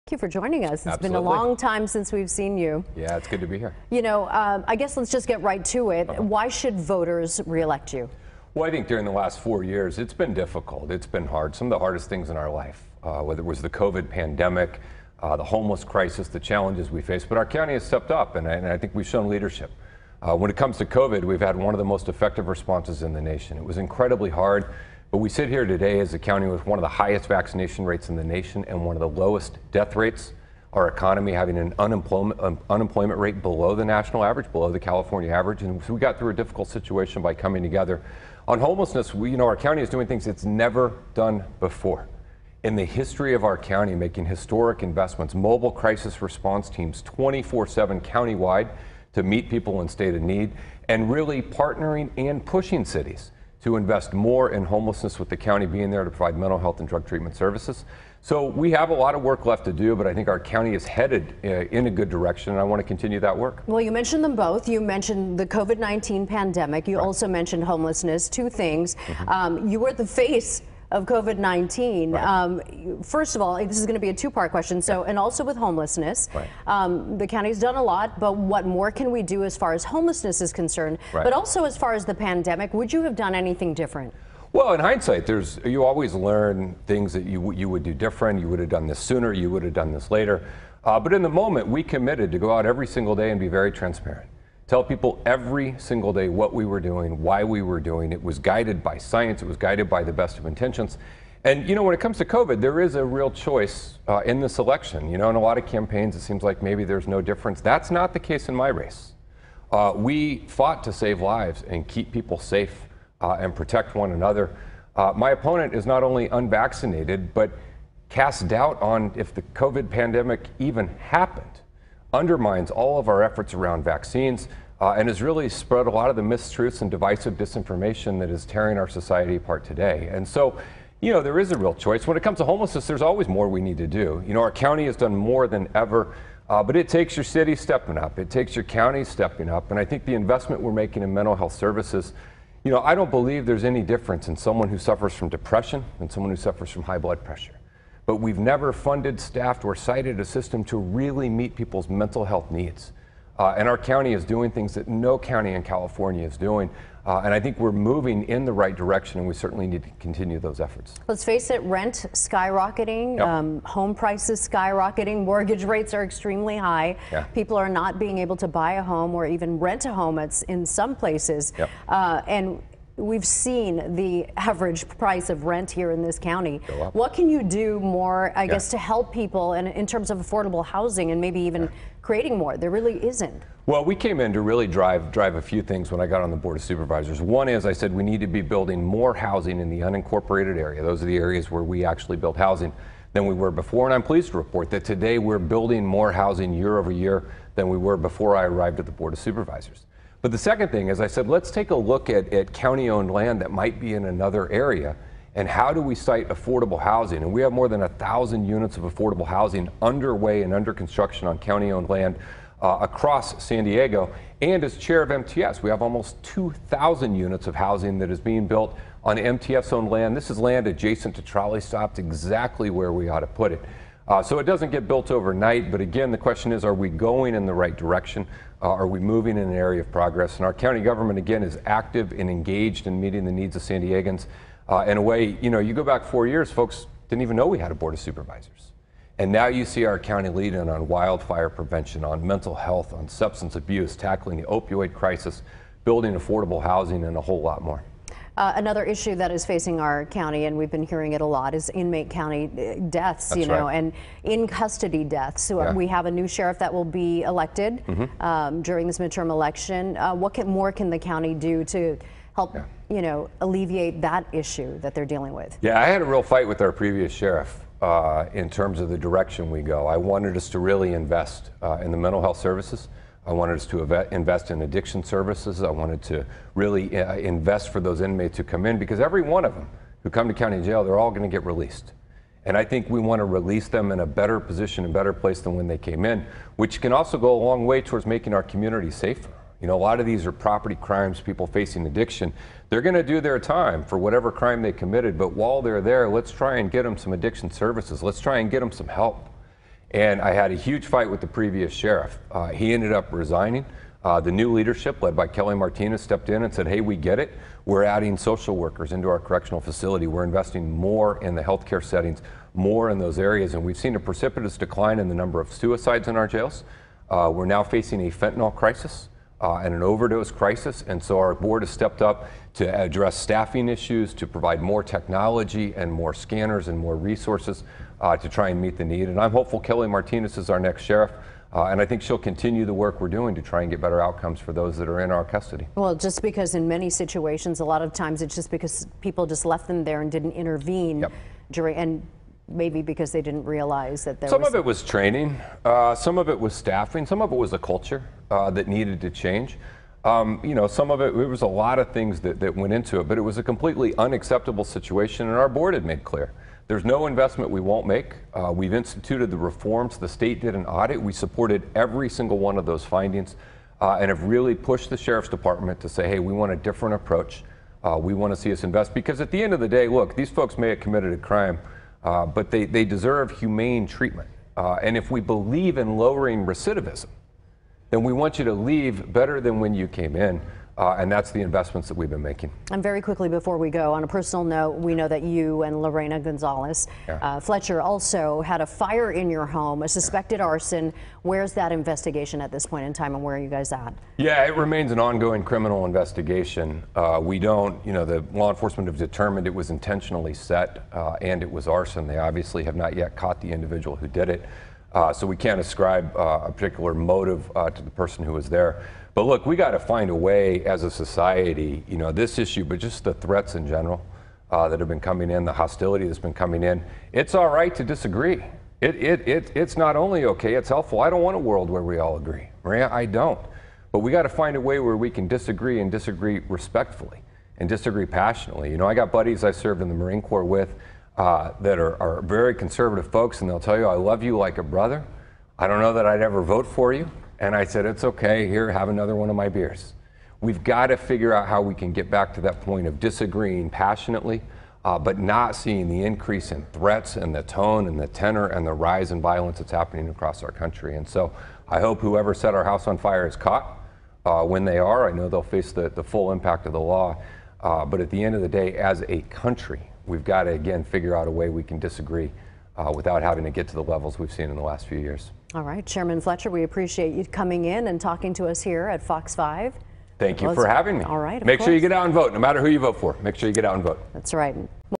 Race for District 4 -- Nathan Fletcher Interview
Nathan Fletcher joins FOX 5 for an interview ahead of Election Day.